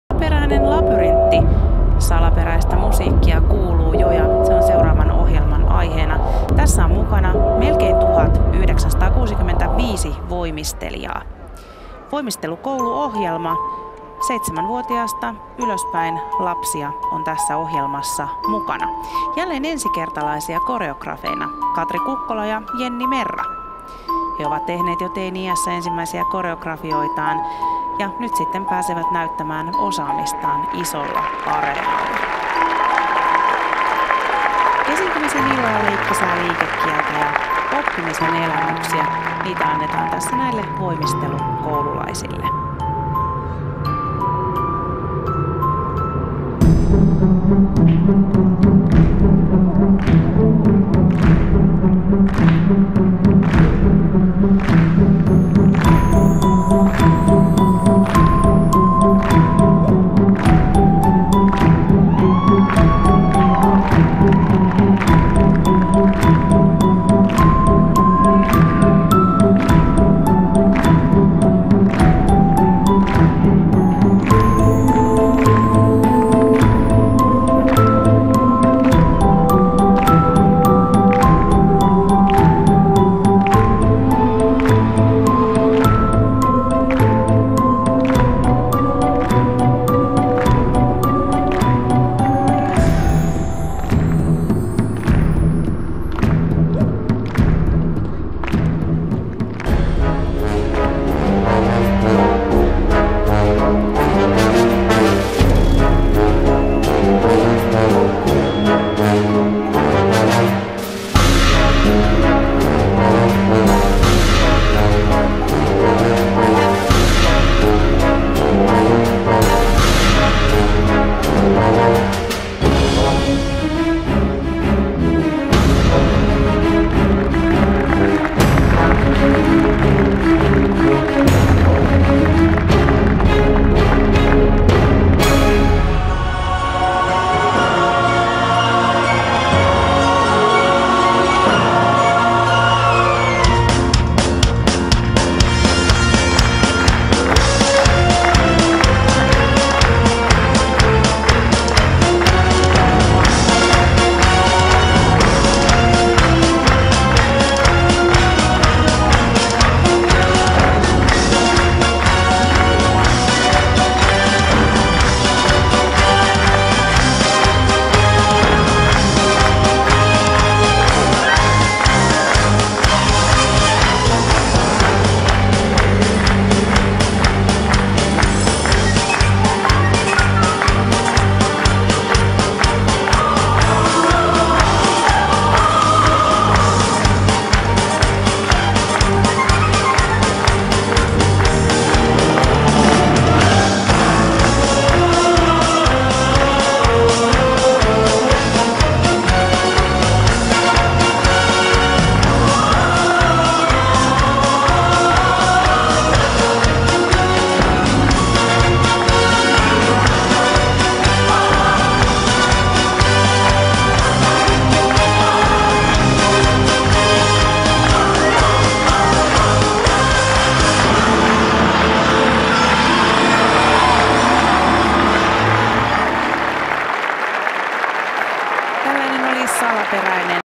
Suomi Gymnaestrada 2018 - Salaperäinen Labyrintti
Suomi Gymnaestradassa esitetty Salaperäinen Labyrintti-näytös sunnuntaina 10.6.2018.
Pääkoreografina toimi Katri Kukkola ja Jenni Merra.
Näytös on esitetty osana sunnuntain päätösjuhlaa.
Yle:n kuvamateriaali.